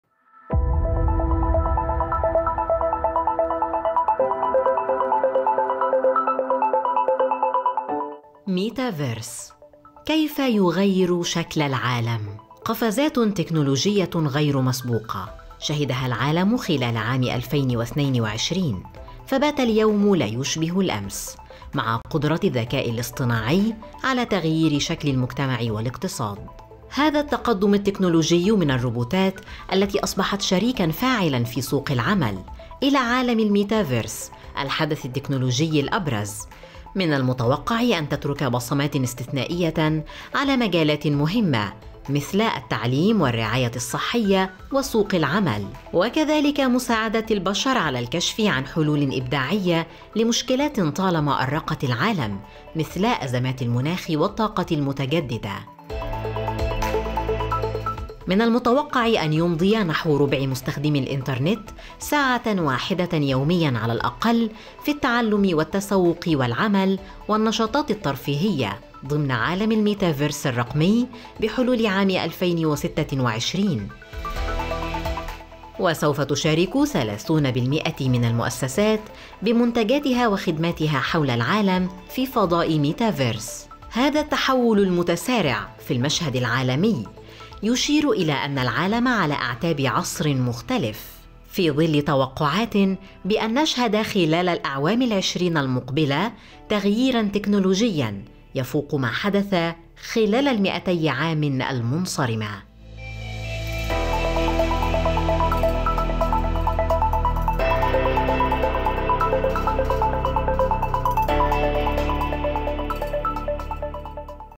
ميتافيرس.. تقنية جديدة تغير شكل العالم| مطروح للنقاش
25/12/2022